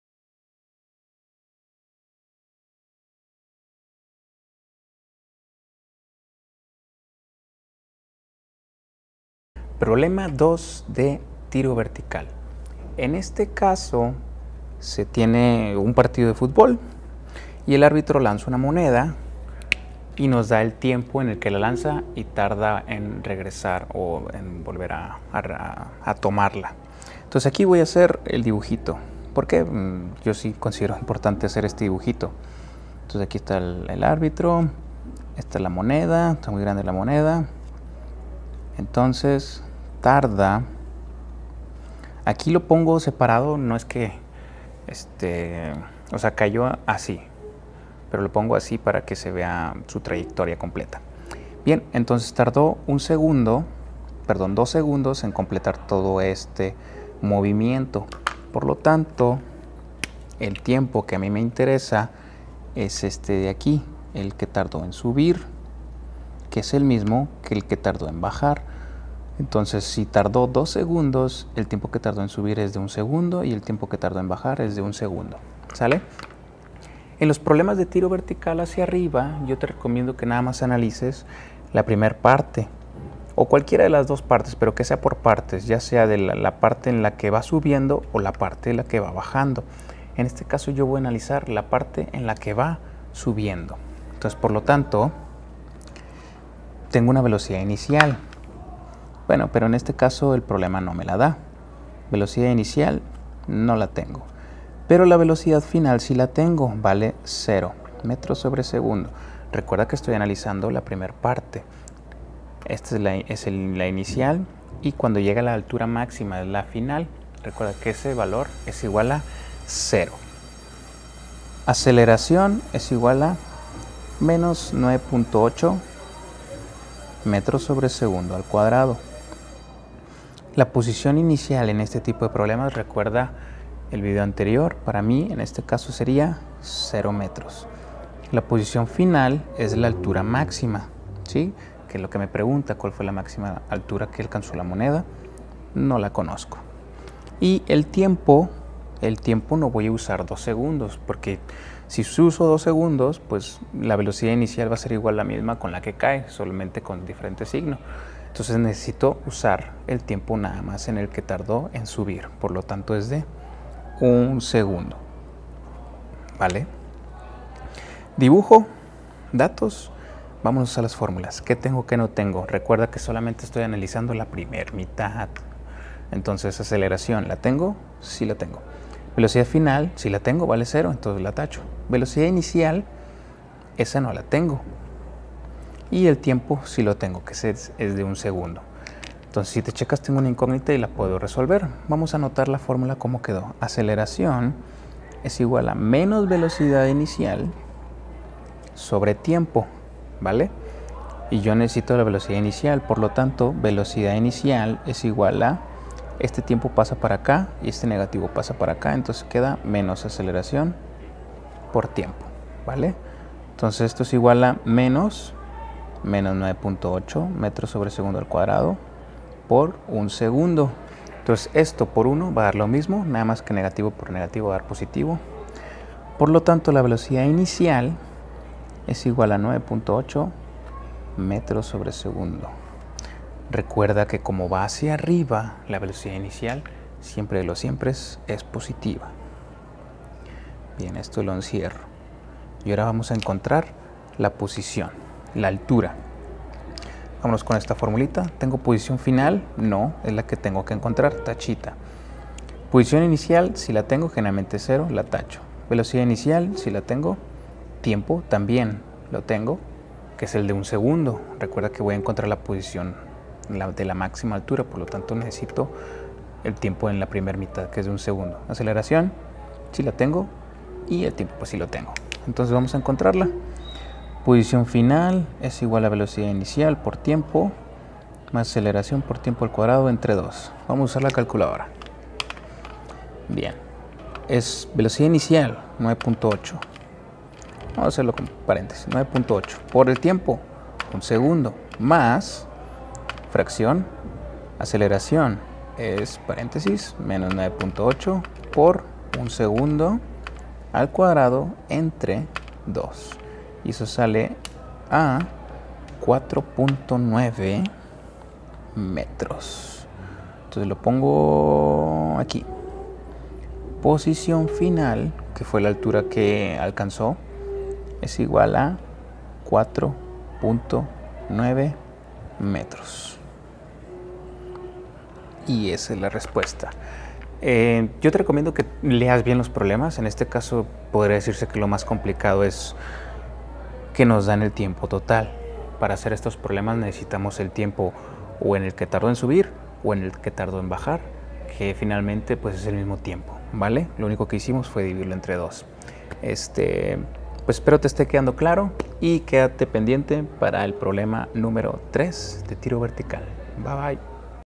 Tiro vertical - Problema 2
Se resuelve el segundo problema de tiro vertical utilizando las ecuaciones de Movimiento Rectilíneo Uniformemente Acelerado (MRUA).